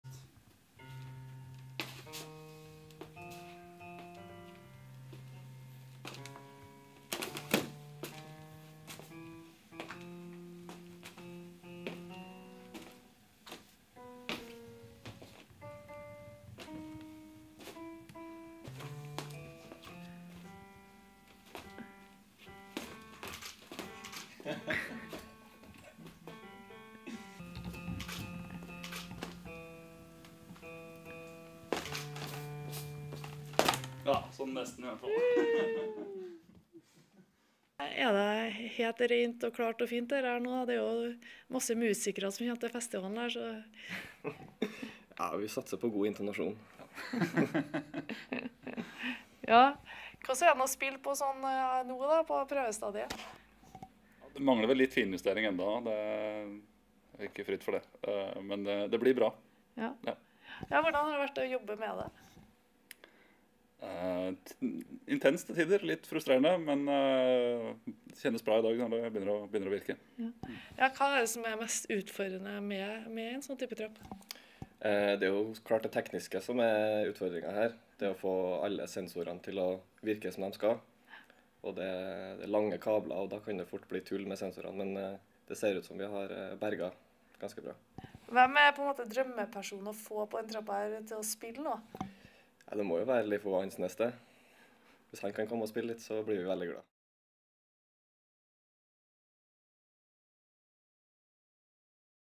Pianotrapp under årets Kammermusikkfestival
Trondheim Chamber Music Festival is always on the search for new ways to perform and show music. This year two students from NTNU have made a piano for staircase. Audience will be able to play on it at Solsiden during the festival.

Her blir pianotrappen til Trondheim kammermusikkfestival spilt på for første gang. Under festivalen blir trappen monternt på Solsiden.